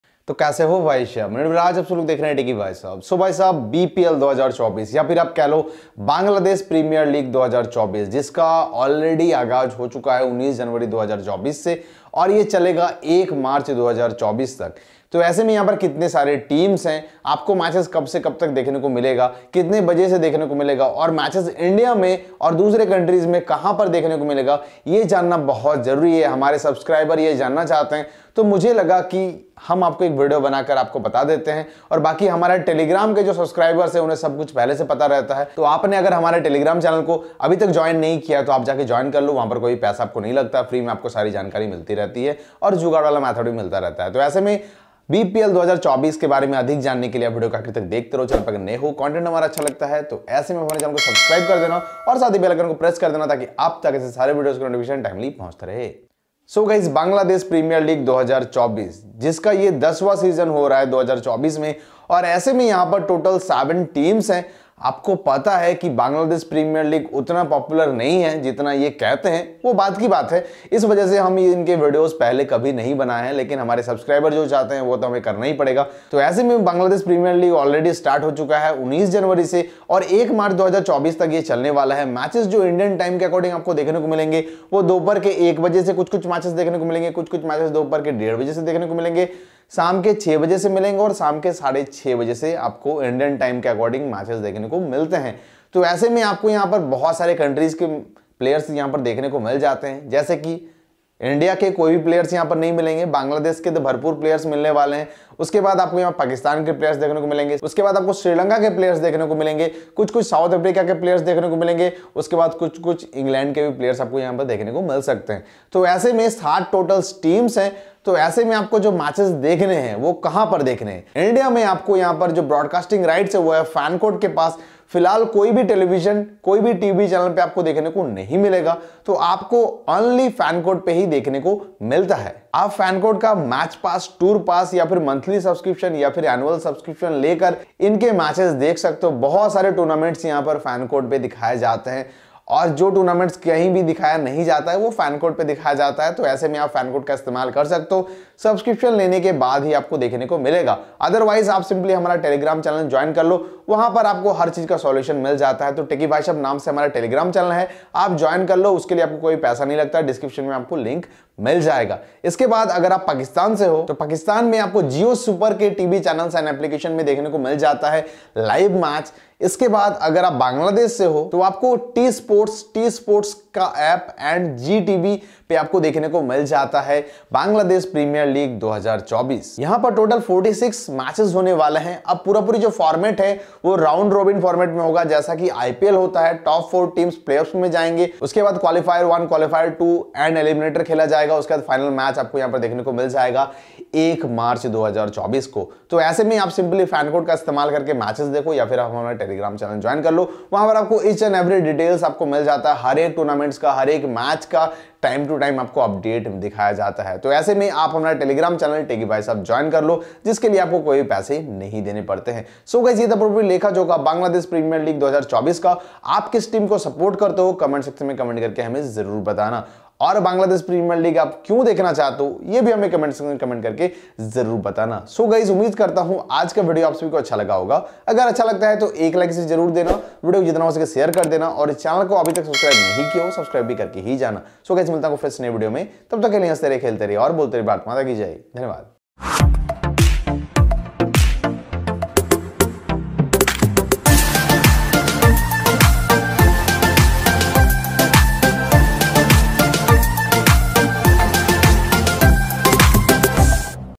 Bangladesh Premier League 2024 Live - BPL 2024 Live Kaise Dekhe
Hello Guys,
In this video i have discussed about Bangladesh Premier League 2024 which is also known as BPL 2024. When and where you can watch the Live matches of BPL 2024 in India, Pakistan & Bangladesh